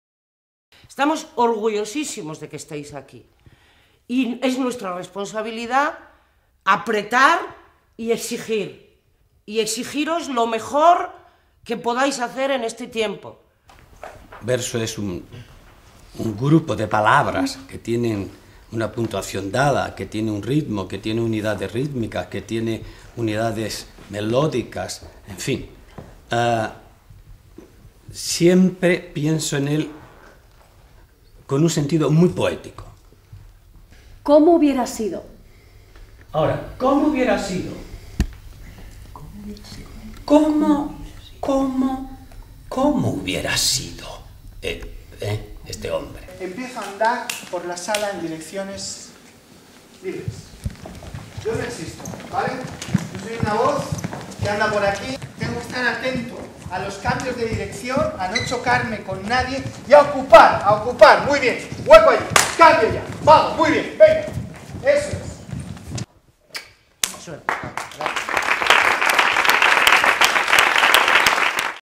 Taller de formación de la Joven Compañía Nacional de Teatro Clásico (2013)
Primera jornada del Taller con los miembros de la Joven compañía Nacional de Teatro clásico.  28 de octubre de 2013 - sala de ensayos de la CNTC. La joven Compañía estrenará dos montajes en la temporada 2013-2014: "El caballero de Olmedo" y "La cortesía de España".